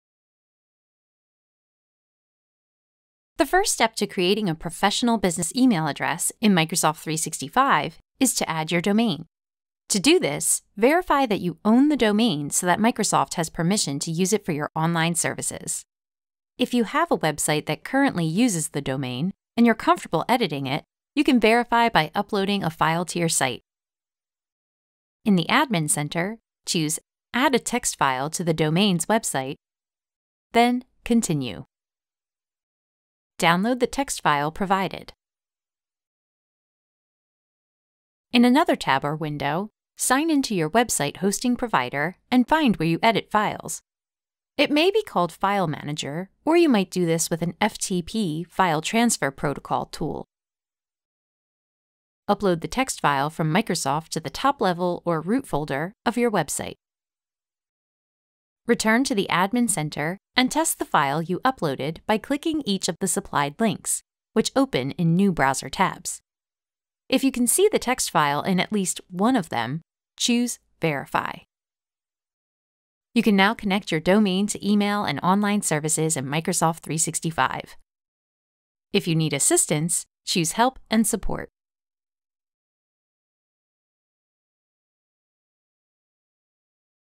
Verify your domain with a text file
Verify that you own the domain you want to use for Microsoft 365 online services by adding a text file to your domain site.